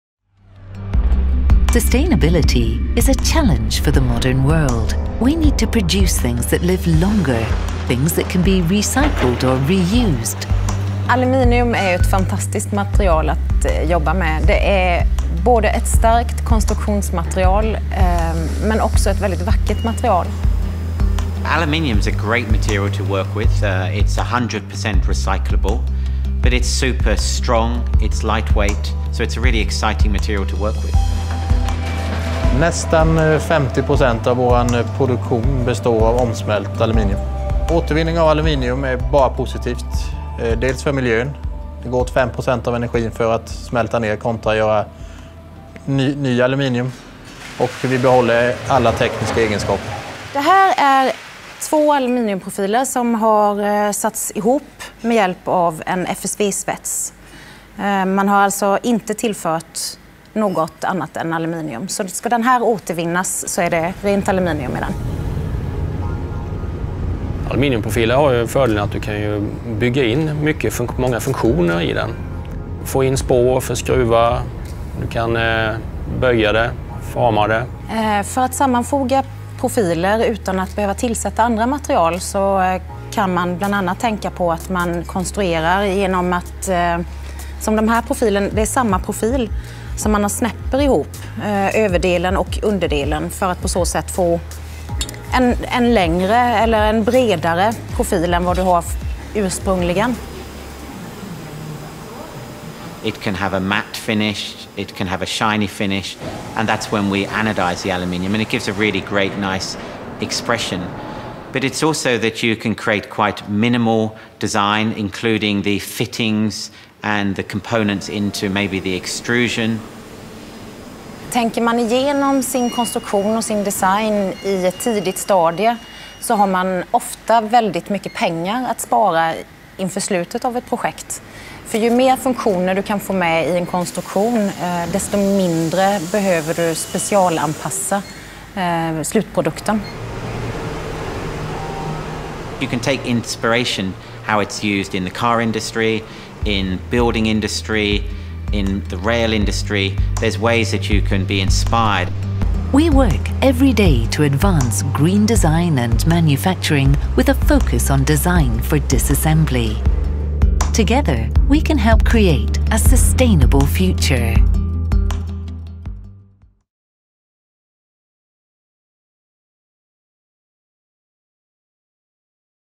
Made by Aluminium - medium version
Documentary about the process of extruding aluminium and how to design products with aluminium (full version).

We are a global supplier of infinite aluminium with activities throughout the value chain, from bauxite to finished aluminium products.